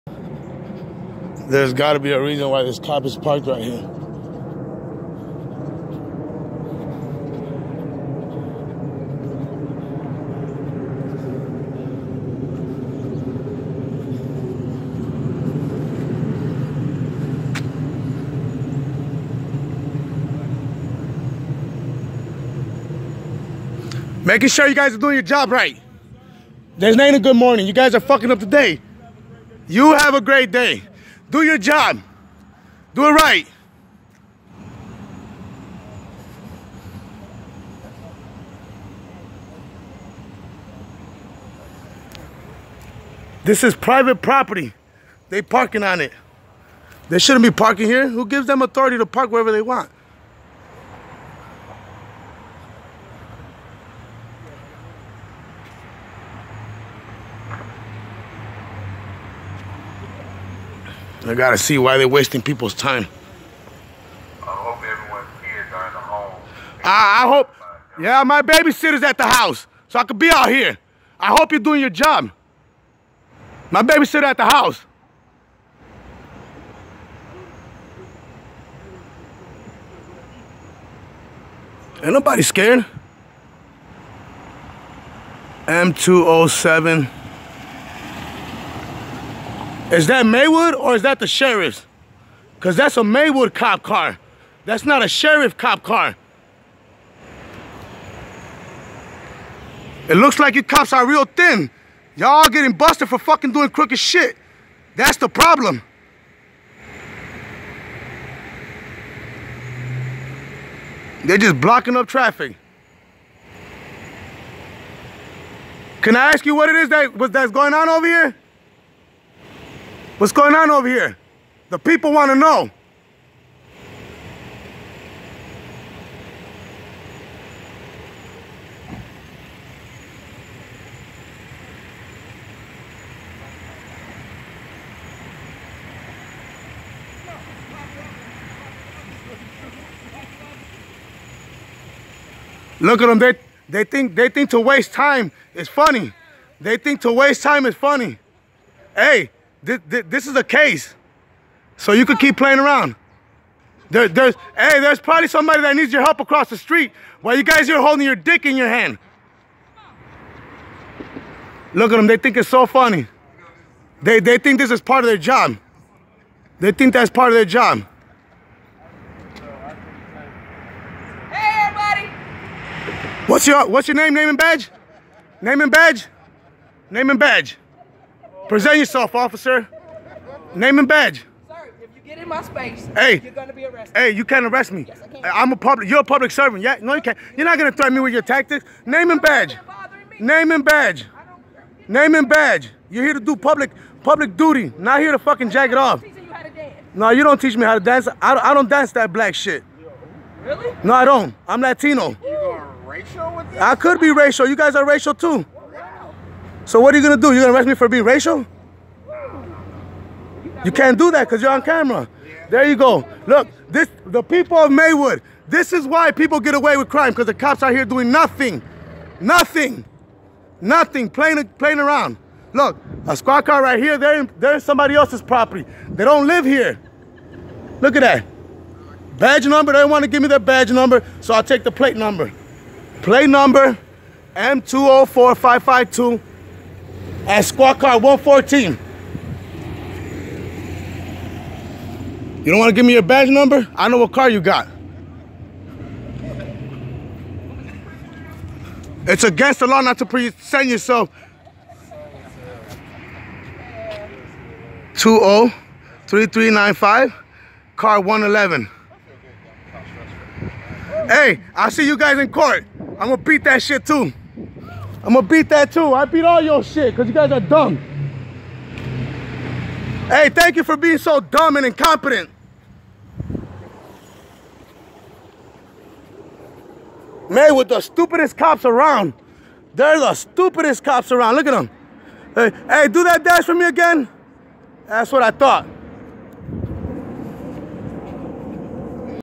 Maywood Police threatens me and thinks their jobs a joke
No professionalism